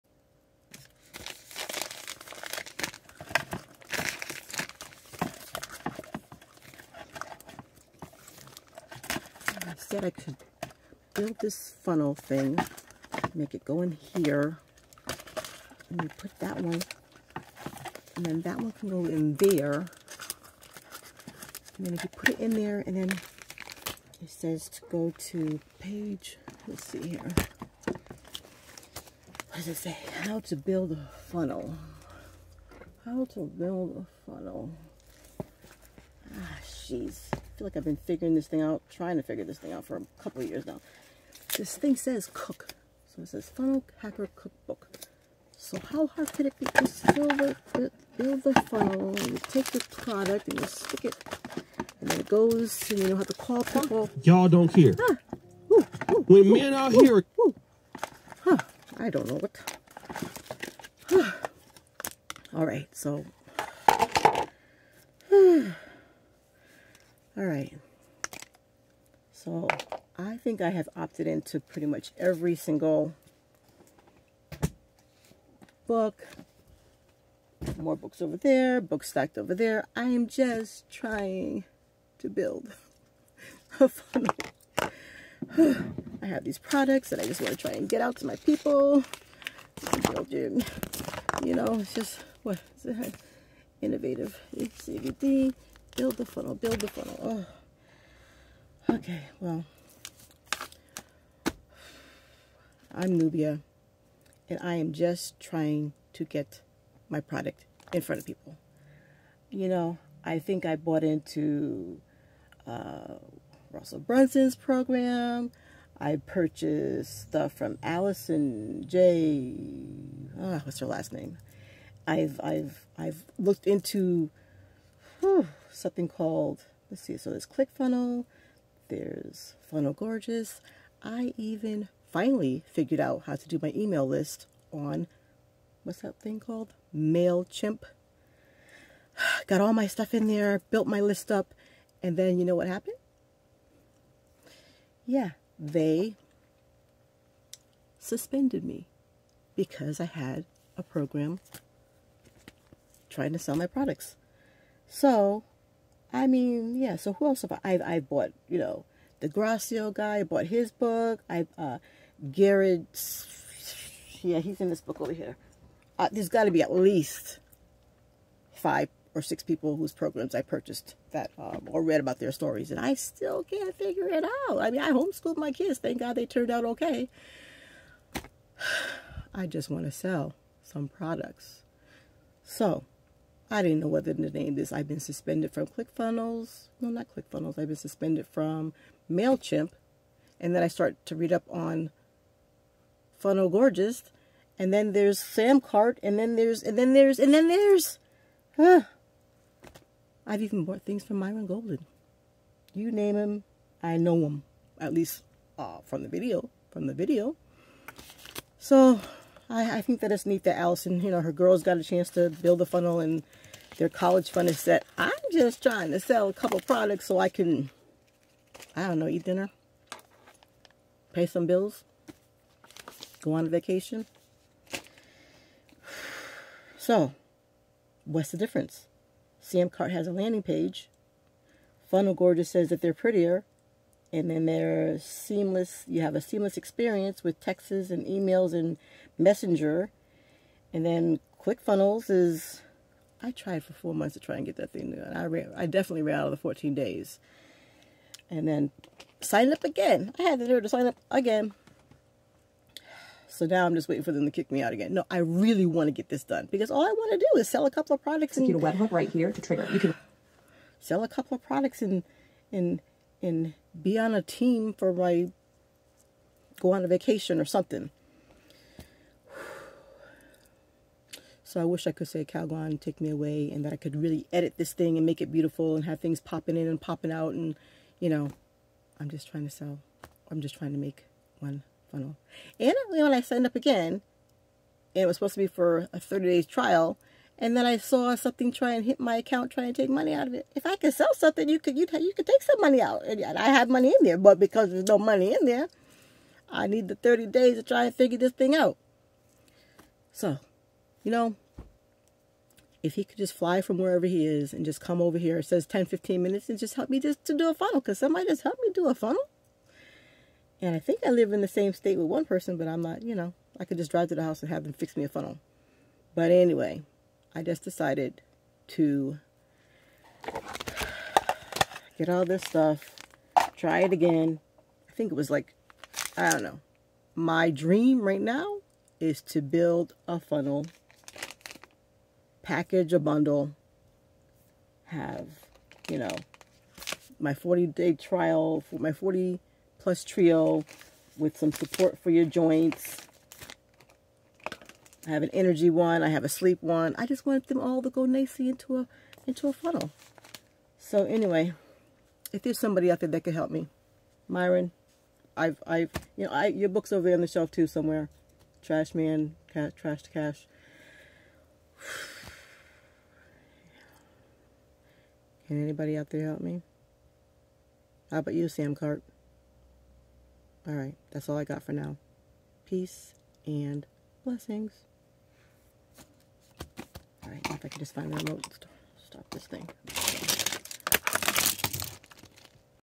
Click funnels vs FunnelGorgeous (HELP)
ClickFunnels vs FunnelGorgeous: Sales funnels and all the intricate details and how-to's. Help, I'm just trying to build a lead magnet and learn how to build a funnel. Can you help me with this service without costing an Arm and a Leg.

How to build a funnel, is the Question? Are you like me and you are still trying to figure out how to make a funnel. join the club me too.

My Goal is to sell enough products to enter into our company challenge and Become a Silver Jacket Club.

PodCast: NubianQueen Nation PodCast